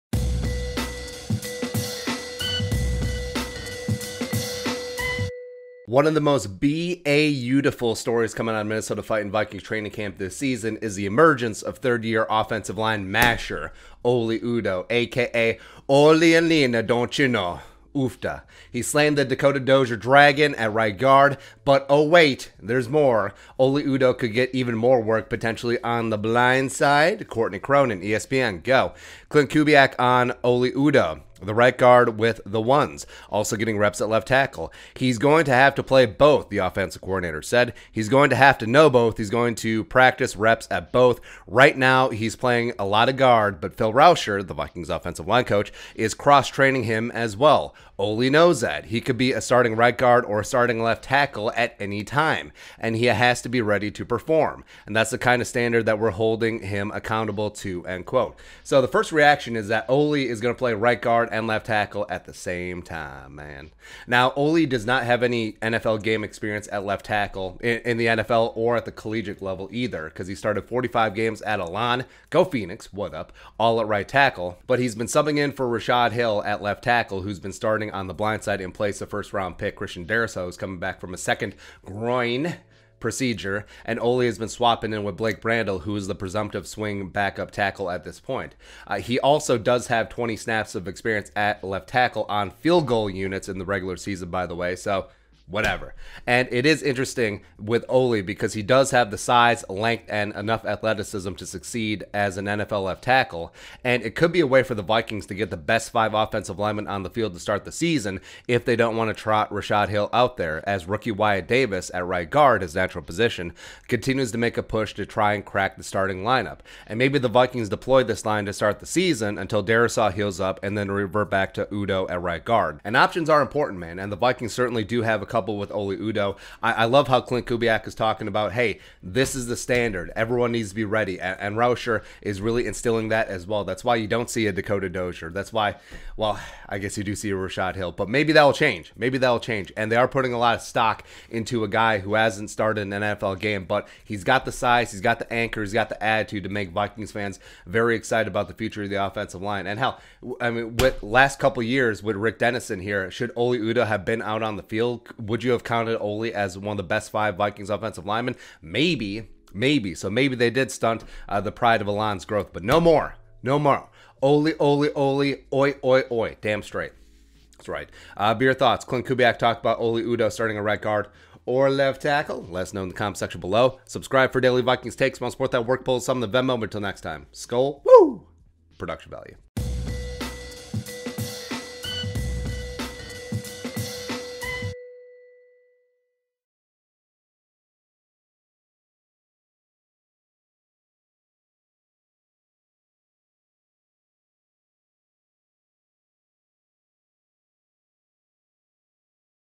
KUBIAK: Oli Udoh Could Start at Right Guard OR Left Tackle 👀👀👀
Minnesota Vikings offensive coordinator Klint Kubiak stated in a press conference that upstart Oli Udoh could start at right guard OR left tackle and needs to be ready to play both.